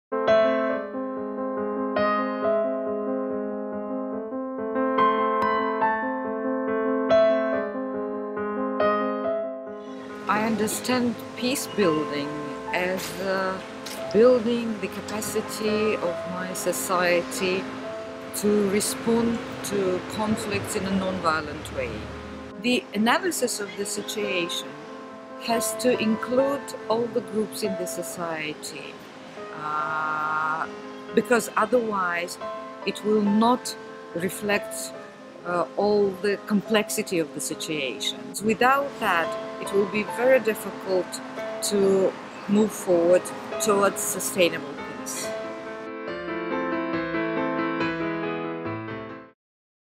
Inclusive peacebuilding is crucial because...
Liana Kvarchelia from our partner The Center for Humanitarian Programmes (CHP) highlights the importance of including all sectors of the society to be able to understand the complexities of armed conflicts and build sustainable peace.